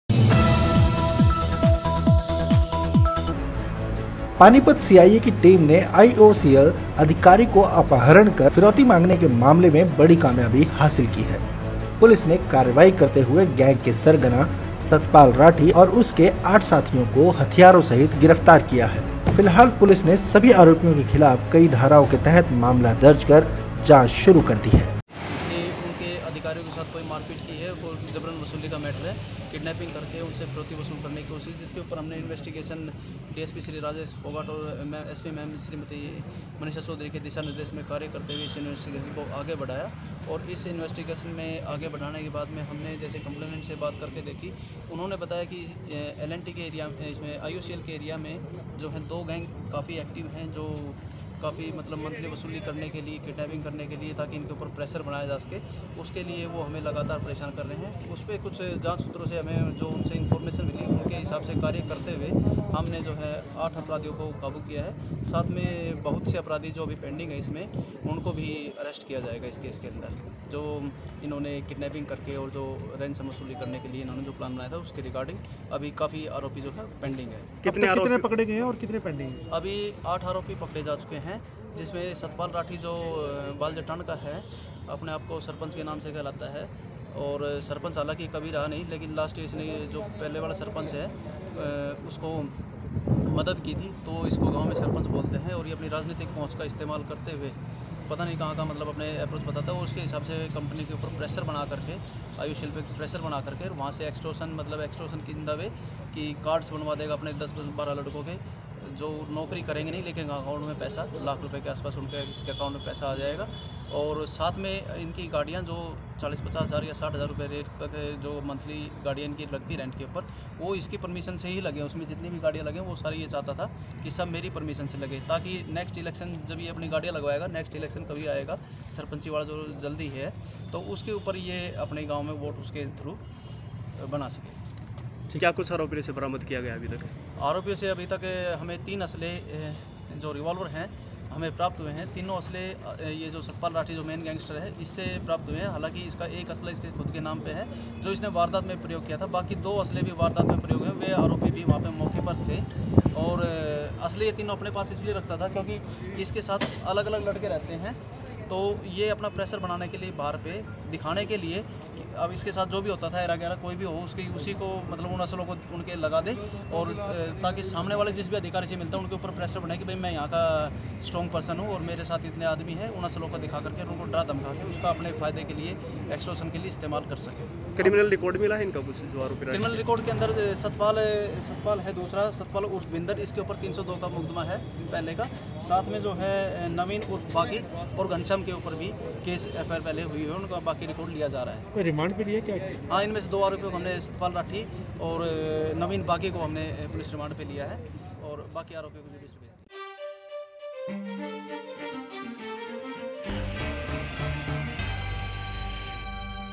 IOCL के अधिकारी का अपहरण करने वाला गैंगस्टर सतपाल राठी 8 साथियों सहित गिरफ्तार
पानीपत (परवीन भारद्धाज)। पानीपत सीआईए टीम की गिरफ्त में नजर आने वाले यह लोग पैसे कमाने के लिए अधिकारियों के अपहरण के दोषी हे। जिन्होंने जल्द पैसे कमाने के लिए तेल कम्पनी के अधिकारियों का अपहरण किया था।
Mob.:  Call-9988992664